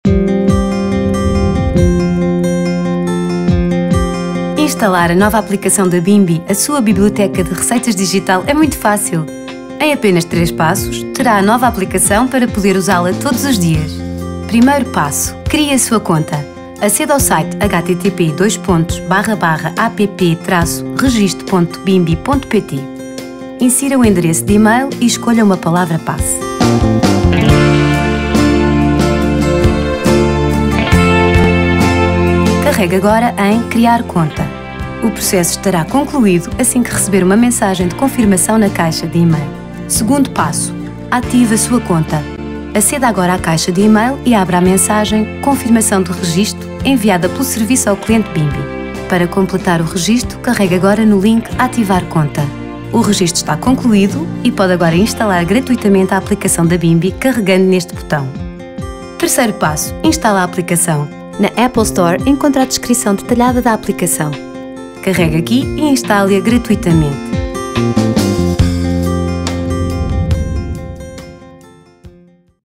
Thermomix -- Bimby - App Oficial da Bimby 2.0 -- Create account and App Download - PT​
Create account and App Download
Discover the new Bimby application "App Oficial da Bimby ® 2.0". Learn how to use your app with our tutorial videos and take the most out of it.

Criar conta e instalar a aplicação
Descubra a nova App Oficial da Bimby ® 2.0. Para desfrutar a 100% a sua aplicação veja os nossos vídeos tutoriais.